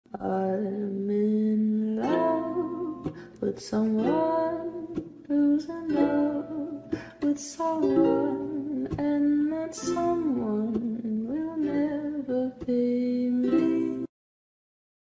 Im in love, with somone....
Akana is in love with Okami, but she thinks Okamis in love with Hikari. Little does she know that she's confusing love with friendship and is driving herself away..